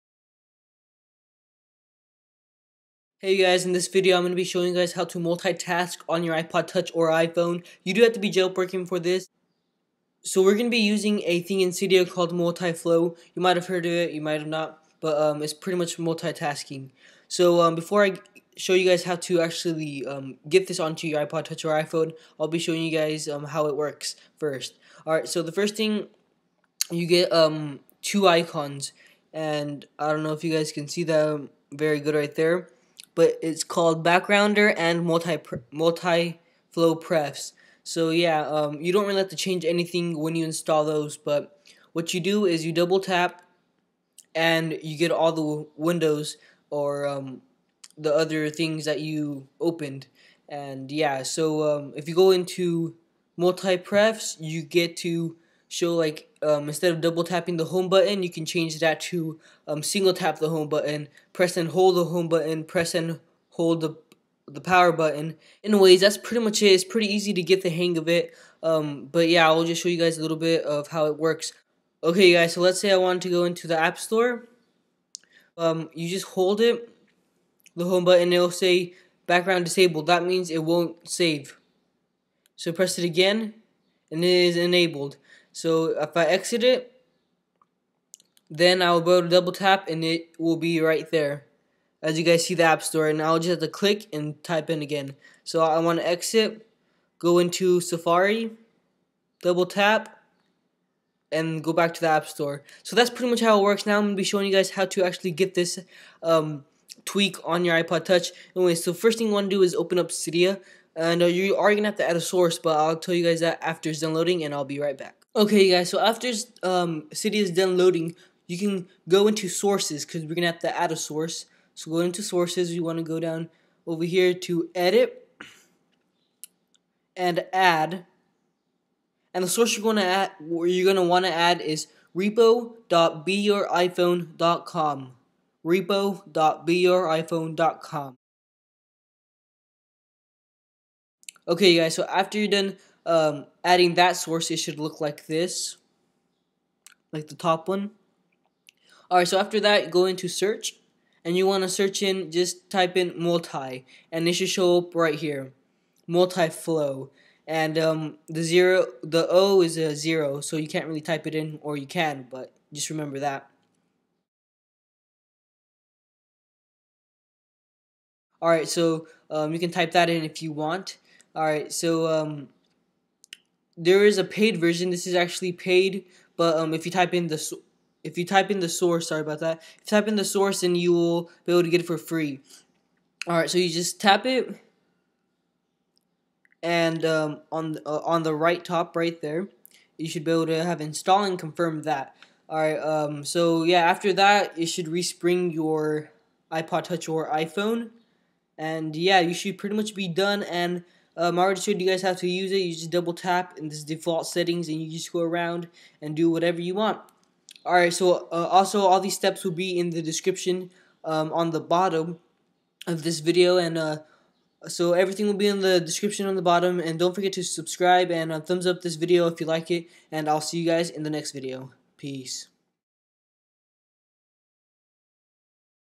How To Multitask on iPod Touch/iPhone!
Hey you guys in this video i show you guys how to Multi-Task on your iPod Touch/iPhone.
Note:(iPod Touch/iPhone needs to be jailbroken)

Steps:
1. Open Cydia
2. Go to Sources
4. Search "Multifl0w"
5. Enjoy